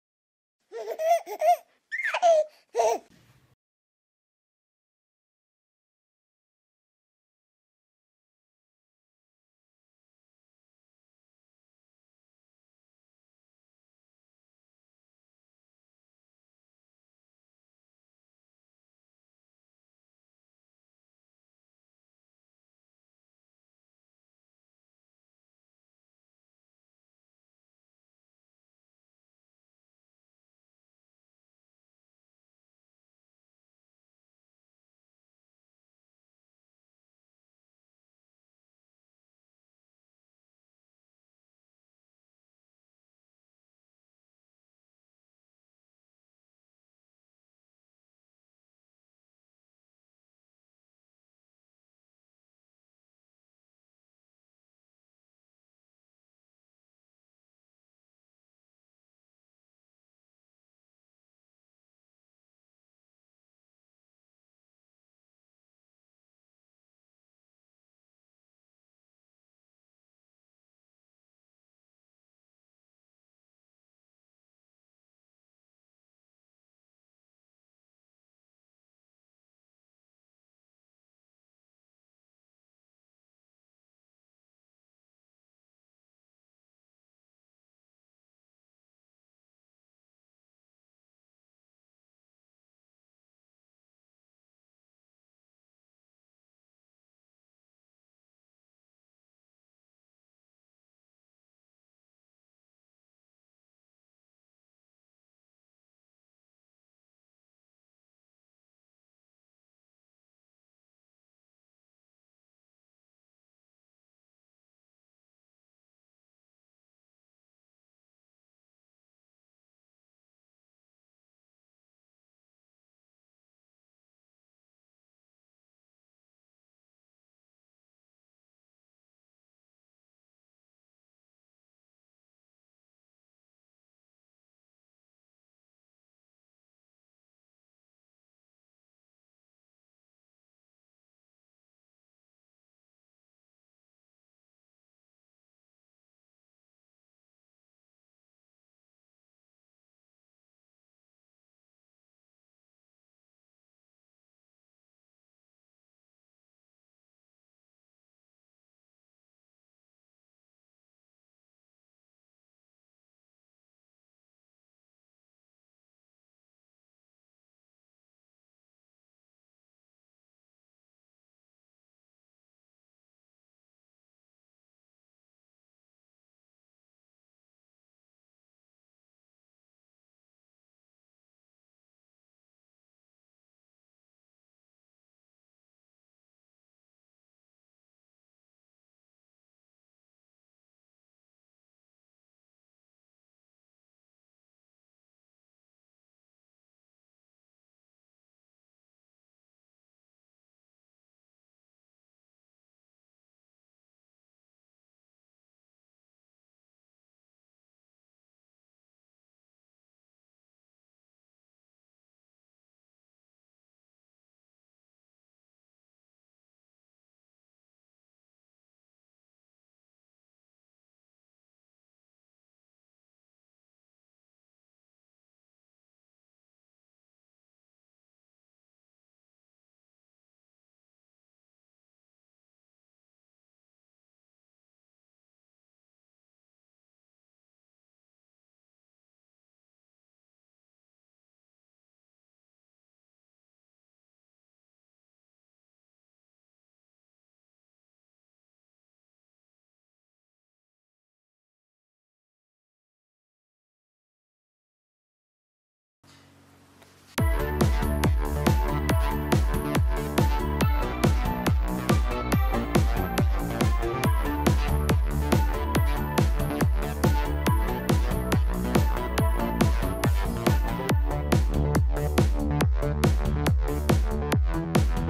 Cats Meeting Babies for the First Time Compilation
When cats meet babies for the first time, you can't tell if they love it or hate it. Either way, their reactions are sure to put a smile on your face!